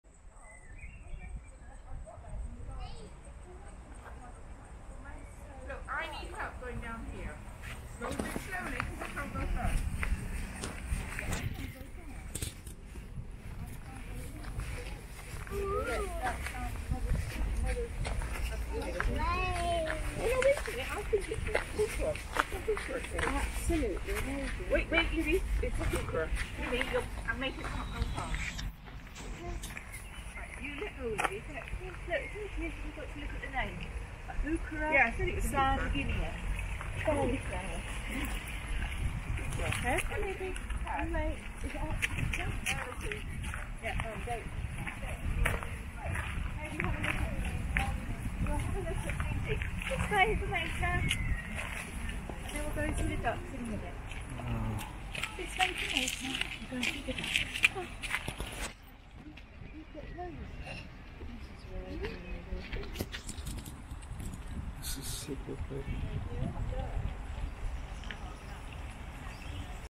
Cambridge Botanical Gardens, UK
One of our first day tours around England, we took a train from London to Cambridge, and one of our first stops was the Botanical Gardens.